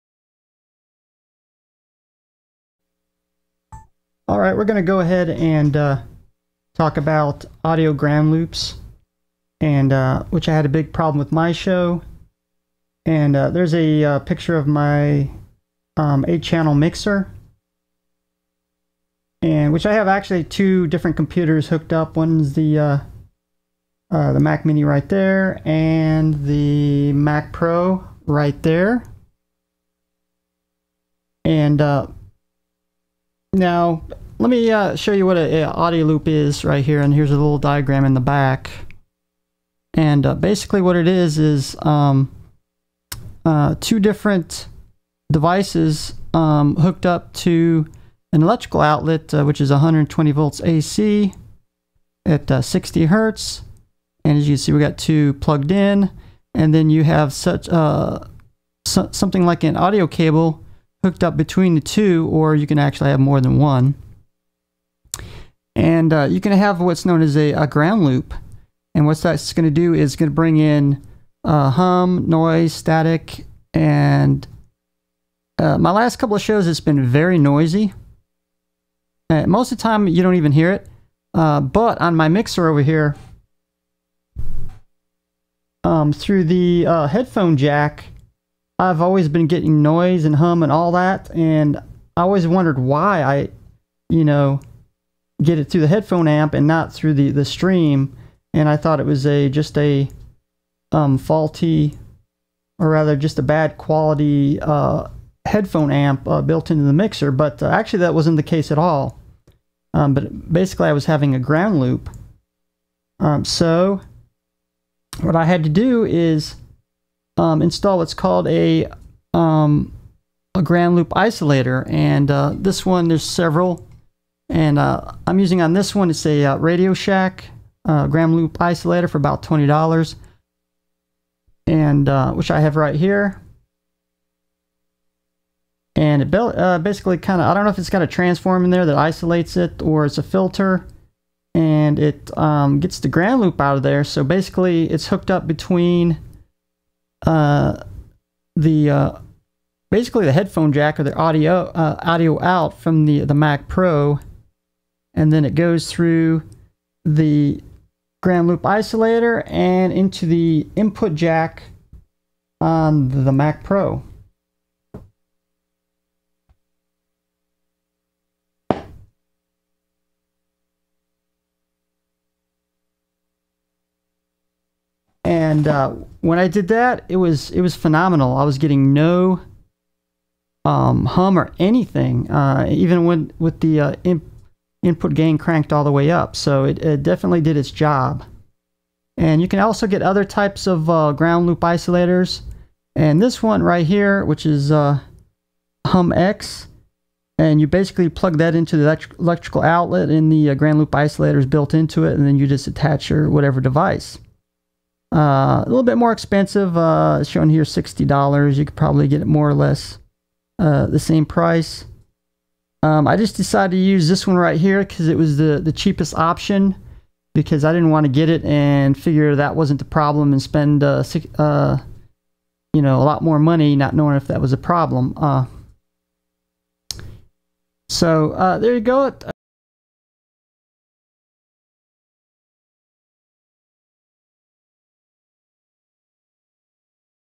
Audio Ground-Loop problems
Tell you about my audio ground loop problem, what it is and how to fix it.